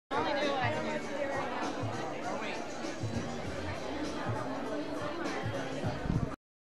Chace Crawford H&M South Beach
Chace Crawford flew in to walk the red carpet and attend the private grand opening party for the new Lincoln Rd H&M store.